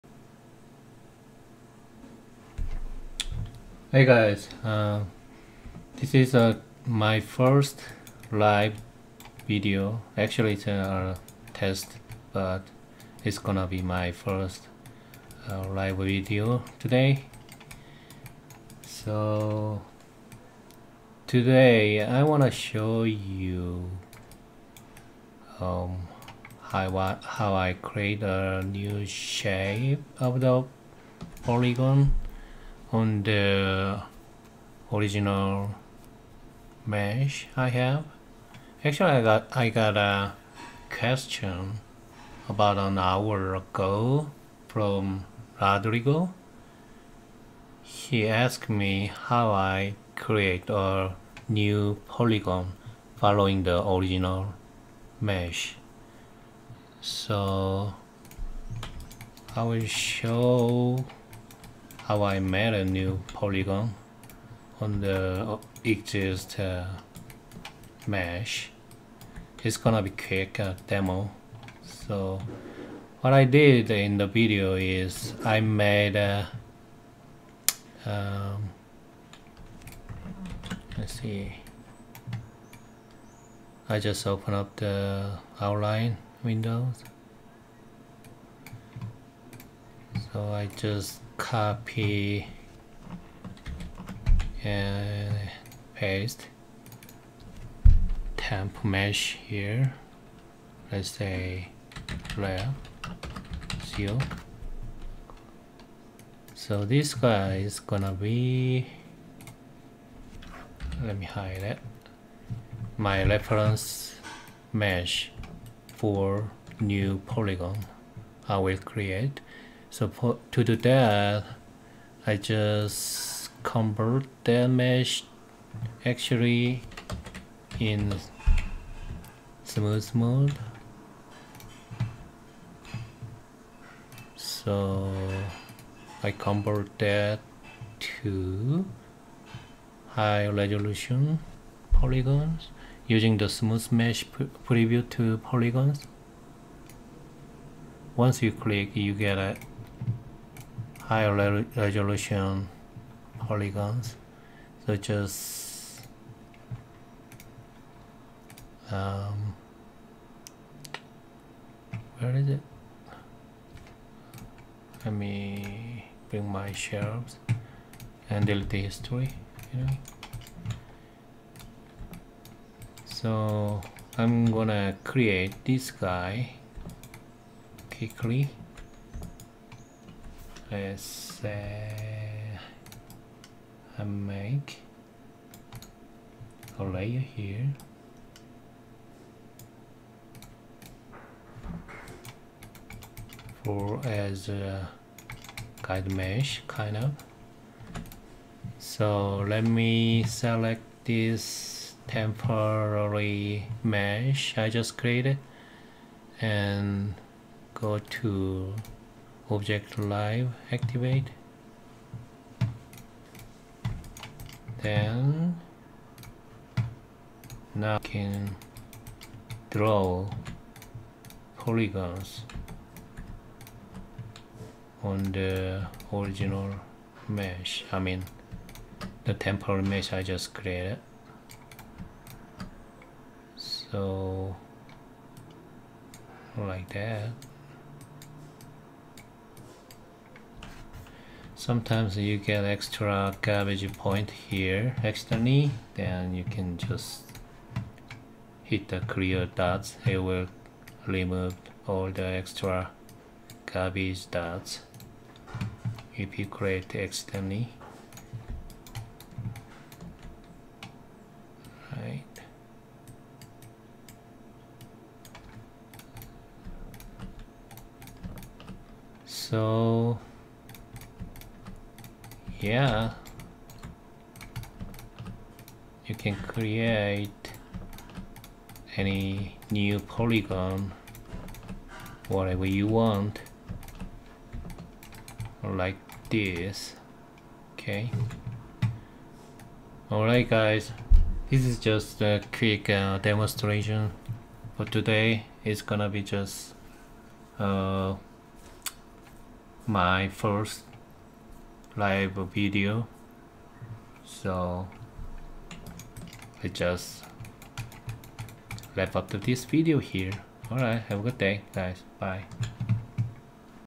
[Test a Live Question] How to use Quad Draw in Maya:
How to use Quad Draw in Maya:

This is a test of live streaming. In this video I will show you how to use  Quad Draw in Maya.